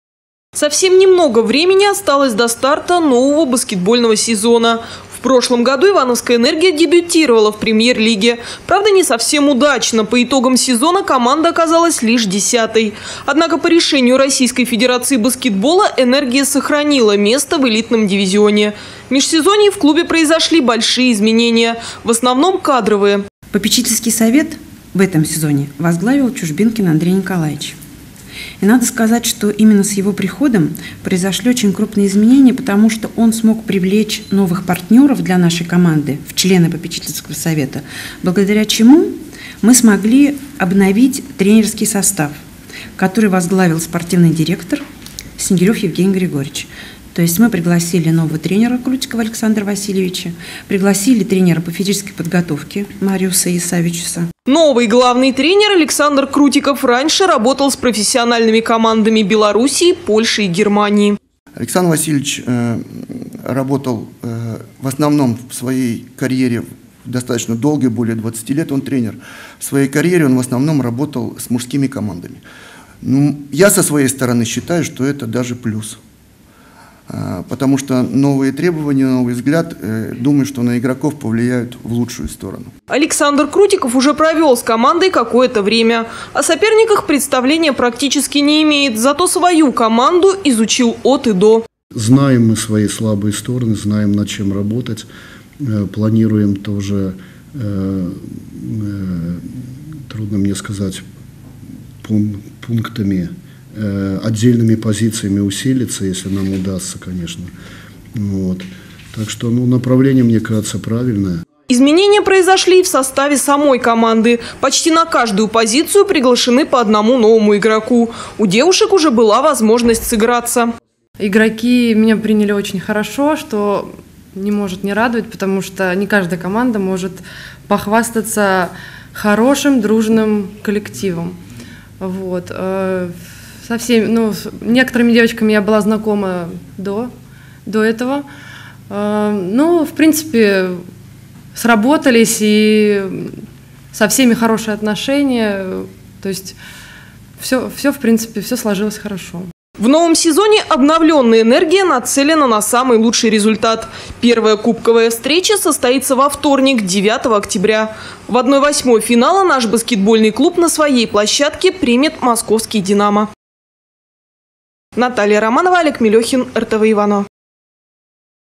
"Энергия" готова к новому сезону в Премьер-лиге
В межсезонье в клубе произошли большие изменения, в основном, кадровые...